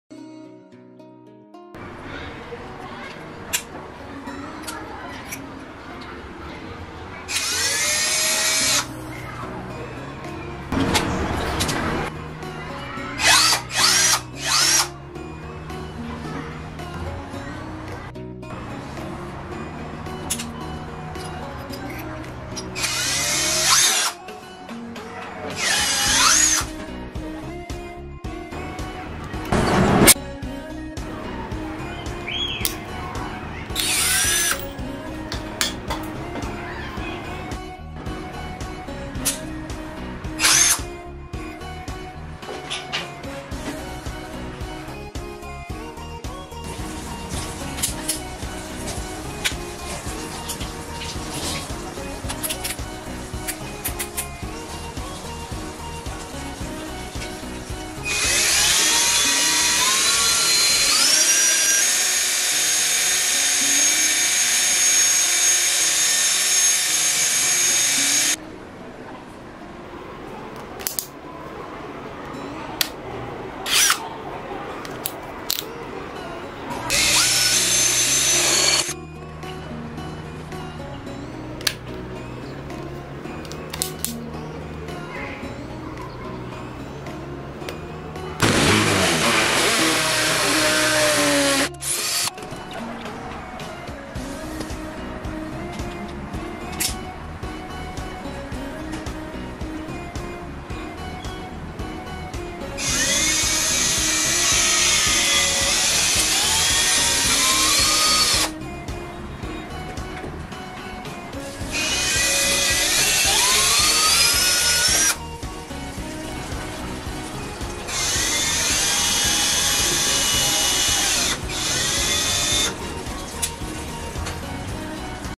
18V枪钻的使用技巧（How to use 18V drill）
视频介绍18V枪钻使用技巧以及各个功能展示（
The video introduces the use skills of 18V gun drill and the display of various functions）